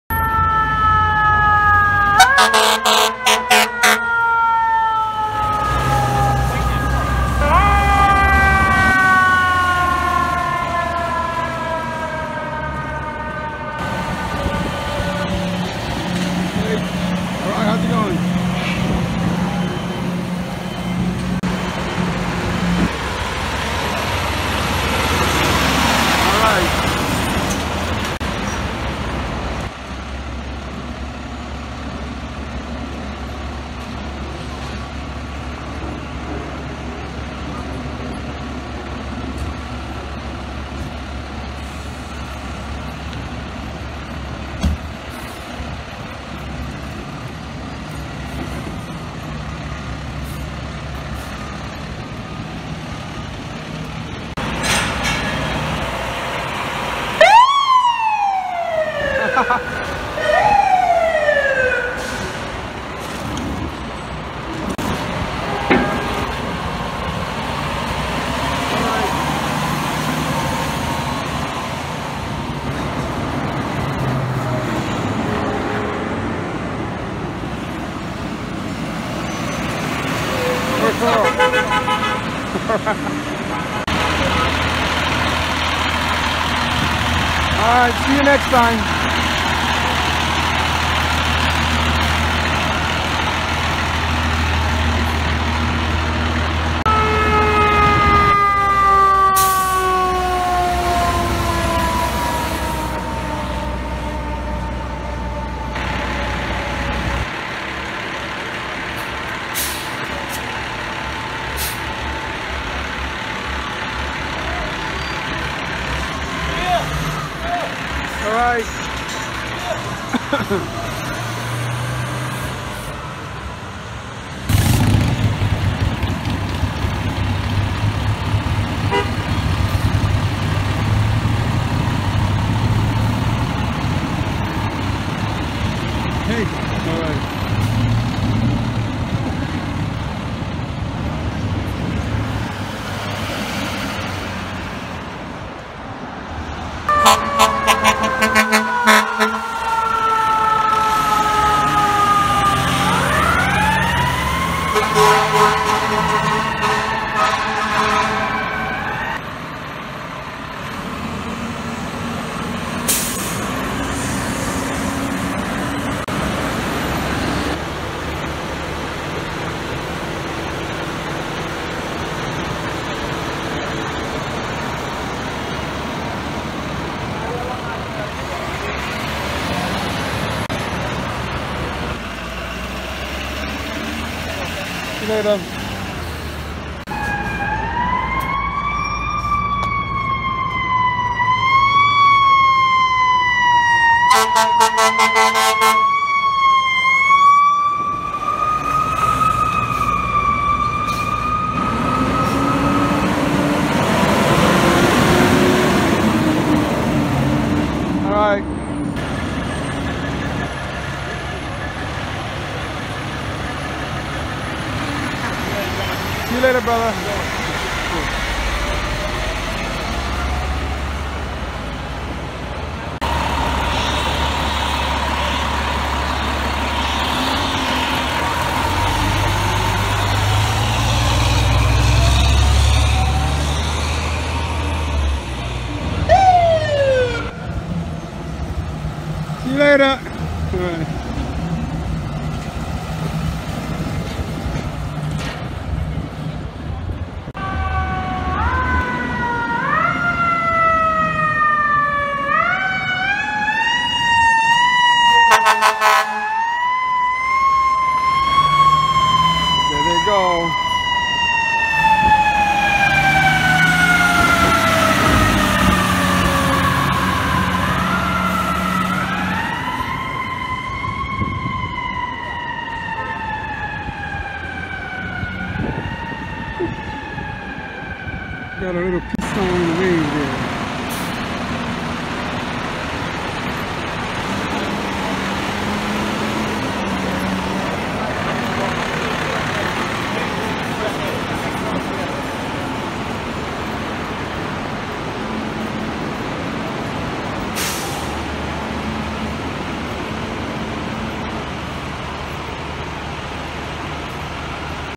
COMPILATION OF FDNY MEMBERS SHOWING ME LOVE & ACKNOWLEDGEMENT WITH TOOTS, WAVES & PEACE SIGNS. 03
HERE YOU WILL SEE A COMPILATION OF FDNY MEMBERS SHOWING ME SOME LOVE WHILE ACKNOWLEDGING ME WITH TOOTS, WAVES AND PEACE SIGNS ON THE STREET OF NEW YORK CITY.  A SPECIAL THANK YOU TO ALL THE MEMBERS OF THE FDNY FAMILY FOR MAKING ME FEEL SPECIAL AND APPRECIATING ME FOR WHAT I DO FOR YOU!  GOD BLESS ALWAYS.